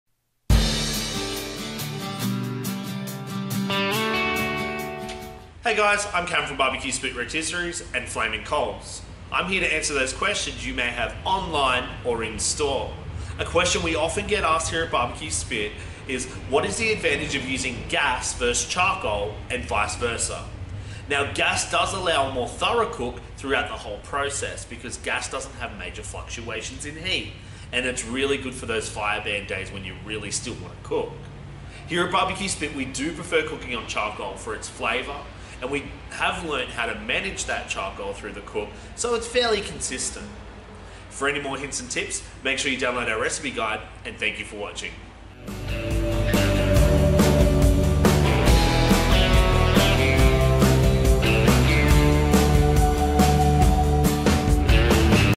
Which is Better - Gas or Charcoal?
Hi Guys, I'm Cam from BBQ Spit Rotisseries and Flaming Coals. I'm here to answer those questions you may have online or in store. In this video, we will talk about the advantage of gas vs charcoal and vice versa.